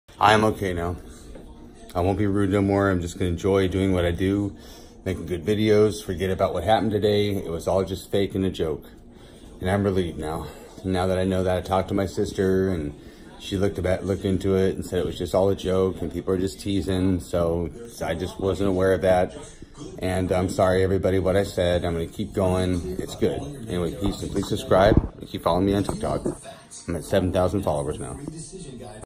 I realize it was just haters today I’m OK and I’m very sorry
Today realizing it was just a couple haters just teasing me a couple trolls I was unaware of and I’m glad that’s over with now I’m good I’m gonna keep going